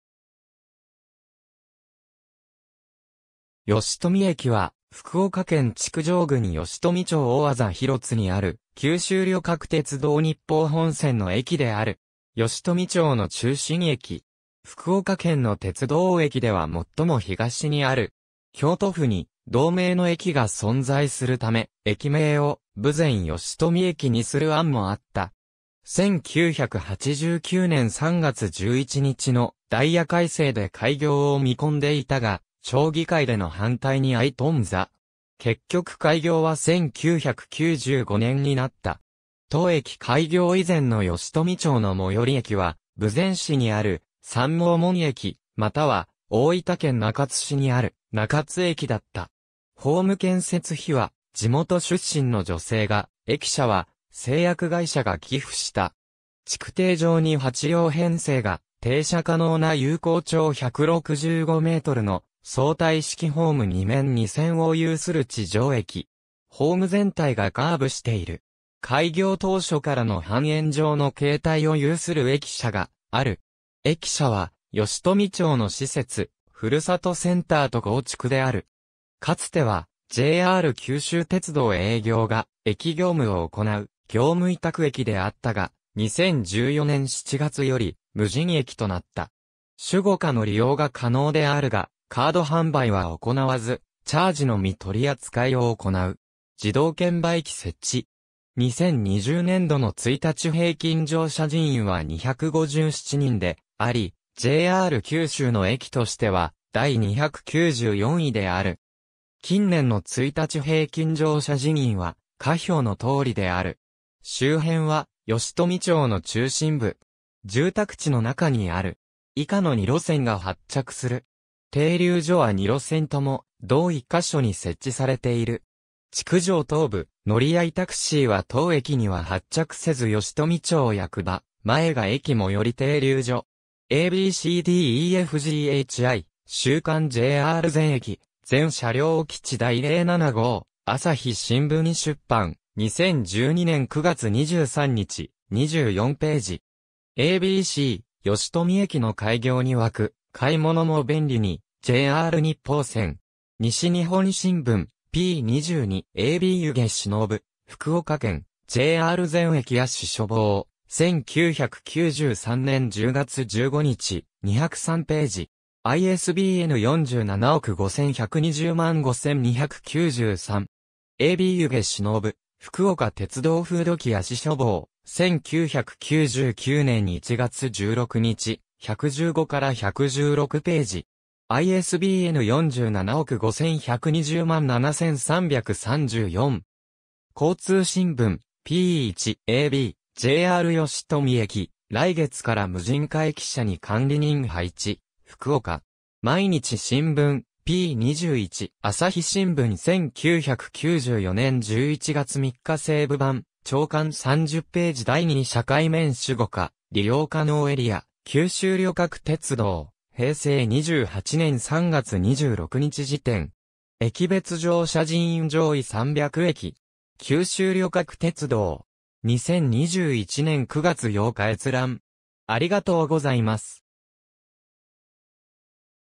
吉富駅 (福岡県)
吉富駅（よしとみえき）は、福岡県築上郡吉富町大字広津にある、九州旅客鉄道（JR九州）日豊本線の駅である。
吉富町の中心駅。
福岡県の鉄道駅では最も東にある。
京都府（山陰本線）に同名の駅が存在する為、駅名を「豊前吉富駅」にする案もあった。
1989年（平成元年）3月11日のダイヤ改正で開業を見込んでいたが、町議会での反対に遭い頓挫。
結局開業は1995年（平成7年）になった。
当駅開業以前の吉富町の最寄駅は、豊前市にある三毛門駅または大分県中津市にある中津駅だった。
ホーム建設費は地元出身の女性が、駅舎は製薬会社が寄付した。
築堤上に8両編成が停車可能な有効長165mの相対式ホーム2面2線を有する地上駅。
ホーム全体がカーブしている。
開業当初からの半円状の形態を有する駅舎がある。
駅舎は吉富町の施設「ふるさとセンター」と合築である。
かつてはJR九州鉄道営業が駅業務を行う業務委託駅であったが、2014年7月より無人駅となった。
SUGOCAの利用が可能であるが、カード販売は行わずチャージのみ取り扱いを行う。
自動券売機設置（オレンジカード使用不可）。
2020年（令和2年）度の1日平均乗車人員は257人であり、JR九州の駅としては第294位である。
近年の1日平均乗車人員は下表のとおりである。
周辺は吉富町の中心部。
住宅地の中にある。
以下の2路線が発着する。
停留所は2路線とも同一箇所に設置されている。
築上東部乗合タクシーは当駅には発着せず吉富町役場前が駅最寄り停留所。
^ a b c d e f g h i 『週刊JR全駅・全車両基地』第07号、朝日新聞出版、2012年9月23日、 24頁。
^ a b c “「吉富駅」の開業に沸く、買い物も便利に・JR日豊線”. 西日本新聞（朝刊） (西日本新聞社): p. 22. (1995年4月21日) ^ a b 弓削信夫『福岡県JR全駅』葦書房、1993年10月15日、203頁。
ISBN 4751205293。
^ a b 弓削信夫『福岡鉄道風土記』葦書房、1999年1月16日、115-116頁。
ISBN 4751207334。
^ 交通新聞 (交通新聞社): p. 1. (2009年3月3日) ^ a b “JR吉富駅：来月から無人化 駅舎に管理人配置／福岡”. 毎日新聞（地方版・福岡） (毎日新聞社): p. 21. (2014年6月6日) ^ 朝日新聞1994年11月3日西部版朝刊　　30ページ第2社会面 ^ SUGOCA 利用可能エリア 九州旅客鉄道、平成28年3月26日時点（2016年10月5日閲覧）。
^ “駅別乗車人員上位300駅（2020年度） (PDF)”. 九州旅客鉄道. 2021年9月8日閲覧。